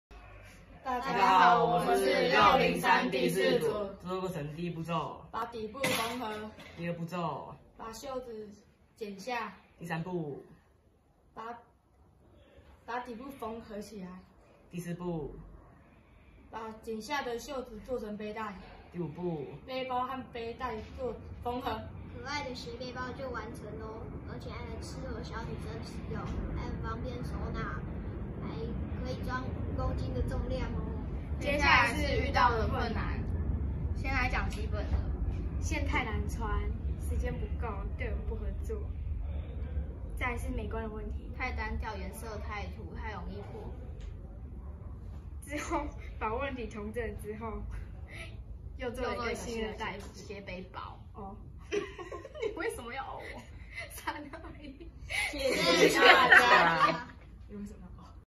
CHI-20200118 - 讓舊衣重生
感受的問題：衣服變多，海洋垃圾太多
輔導員： 陳瑋婷
小推手：
簡俊宇
鍾嘉祐
林育樂
廖姿嘉
戴卉妤